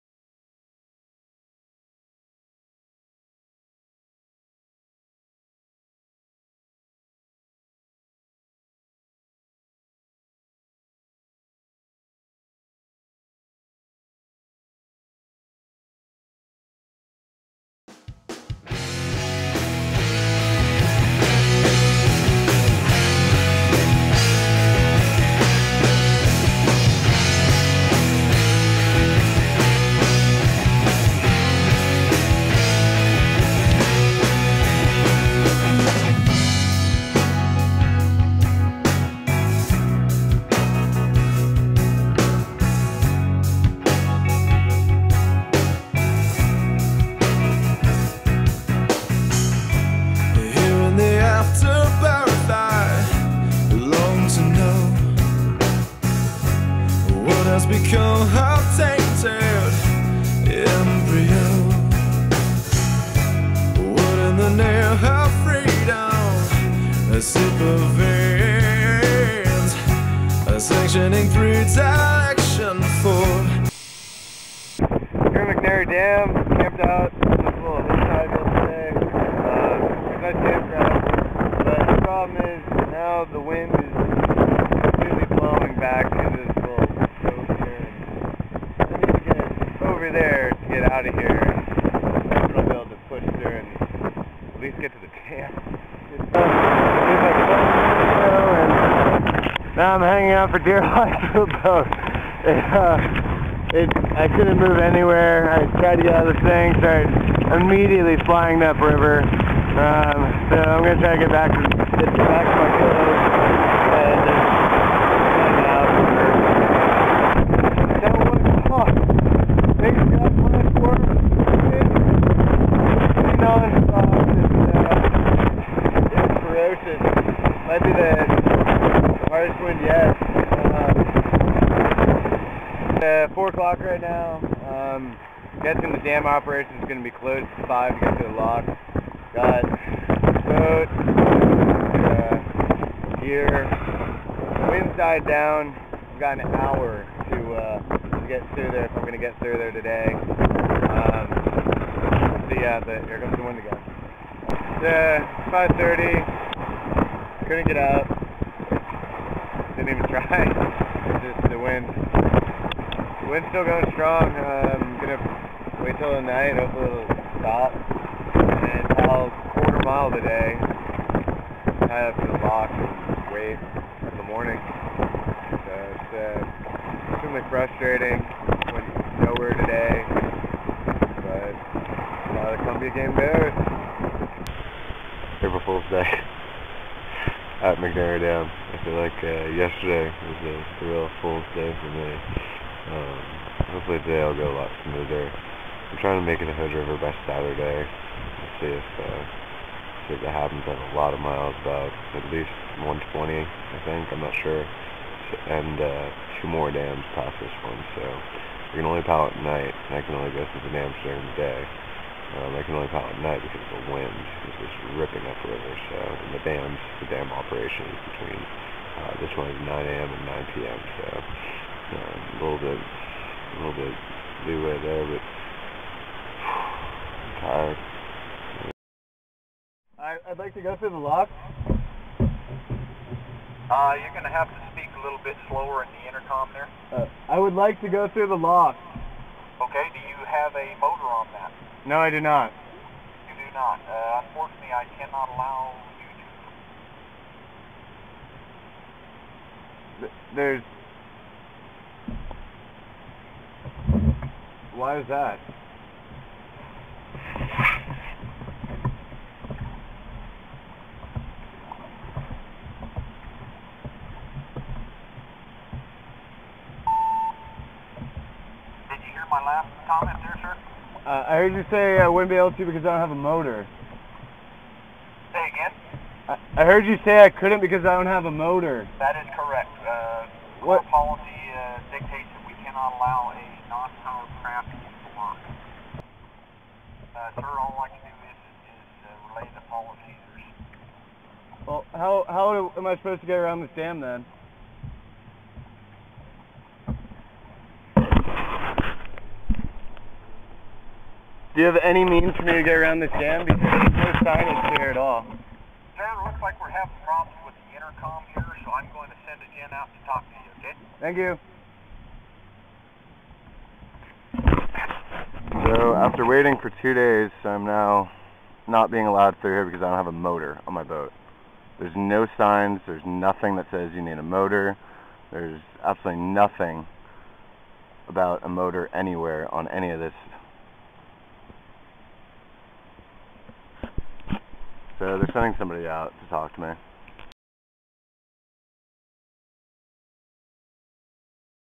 TCE - Kenniwick to McNary Dam
After a nice break in Kennewick, McNary Dam proved to be a bigger obstacle than expected!